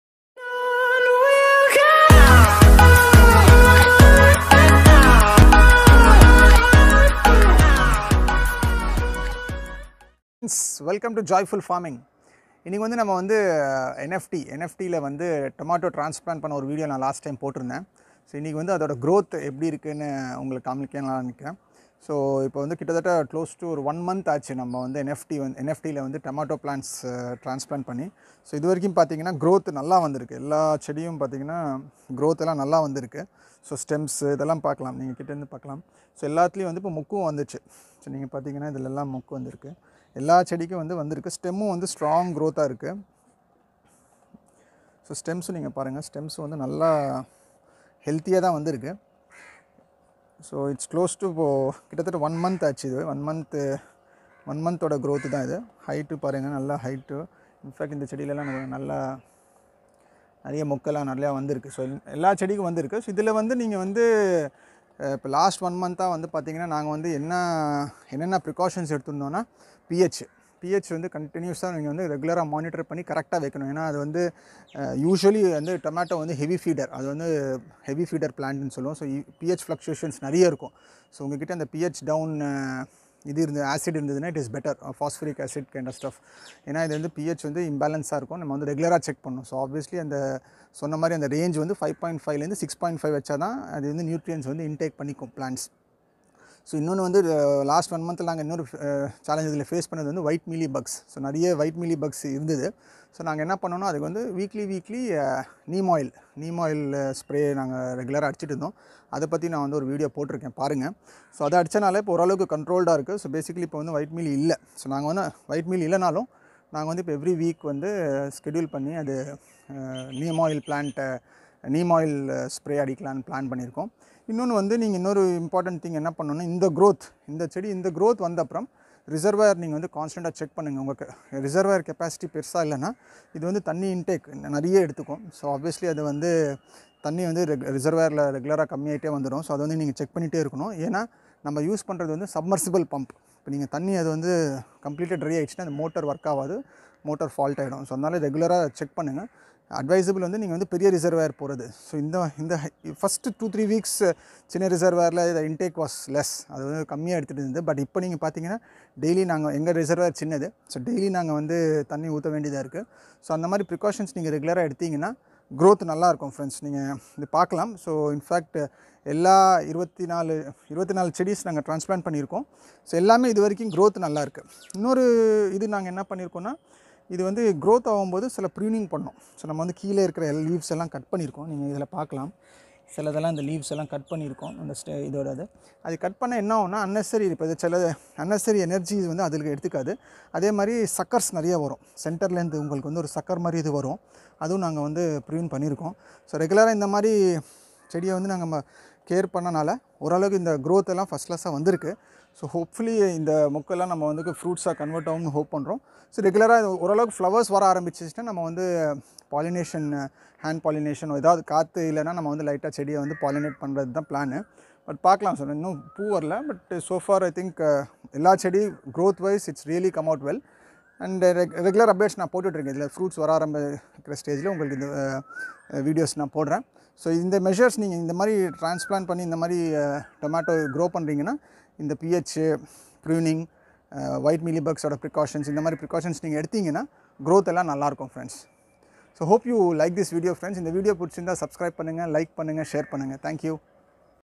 Hydroponic Tomatoes|NFT Tomatoes|Joyful Farming
In this video we show the growth of tomatoes in NFT after 4 weeks of transplantation. NFT Hydroponic tomatoes